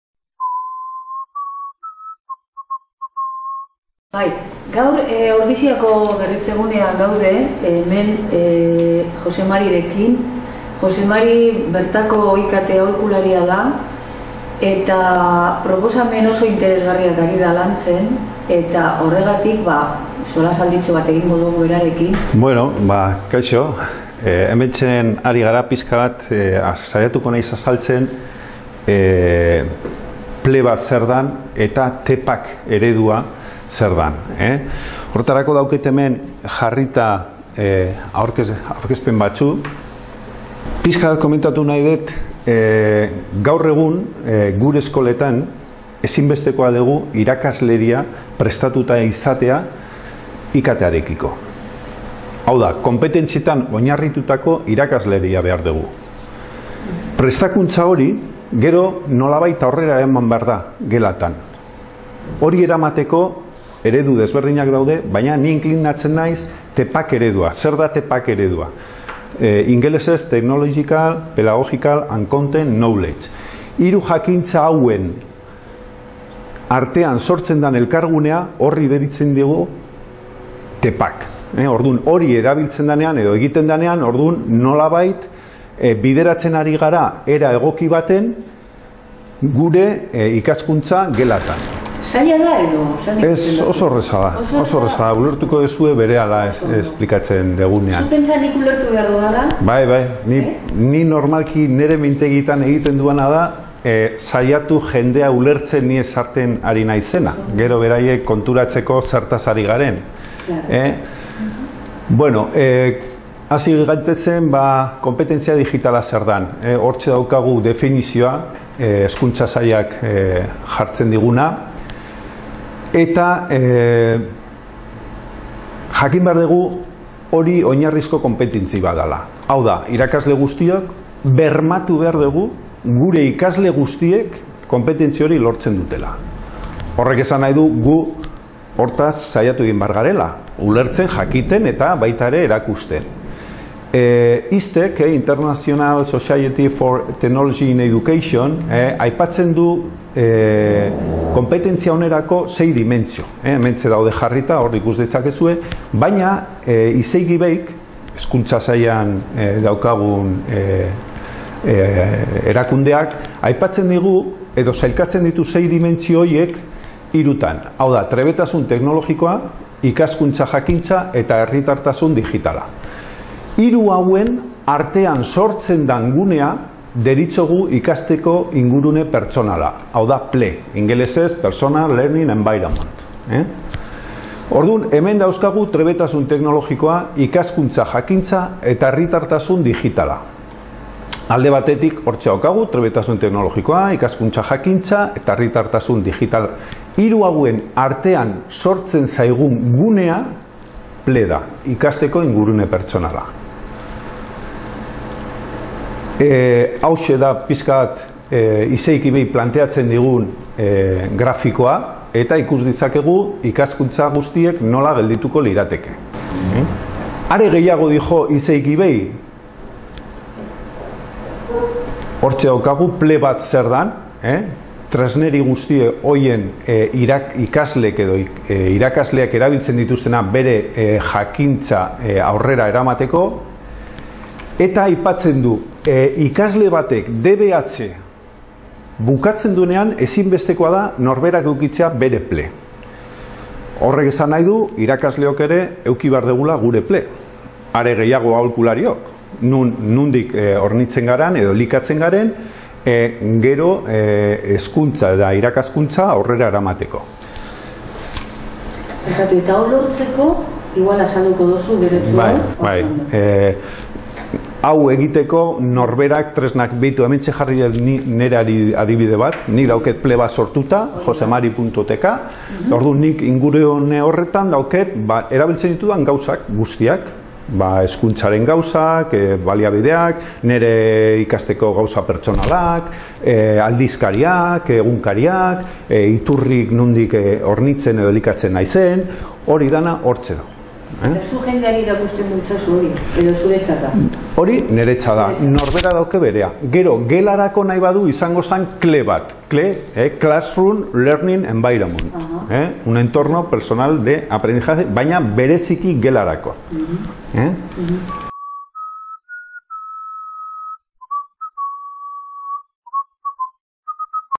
PLE & TPACK
Jose Mari de Juan Fonseca Ordiziako berritzeguneko IKT aholkulariak TPACK eta PLE zer diren azaltzen dio Lorea Aretxaga aholkulariari